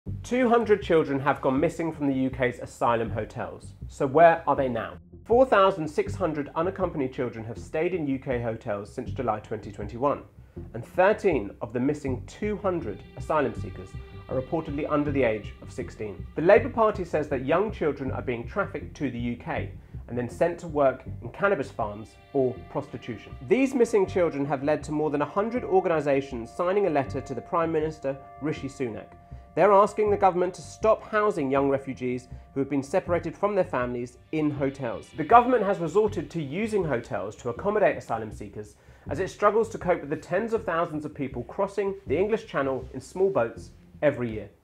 200 children have gone missing from the UK’s asylum hotels
200 children have gone missing from the UK’s asylum hotels.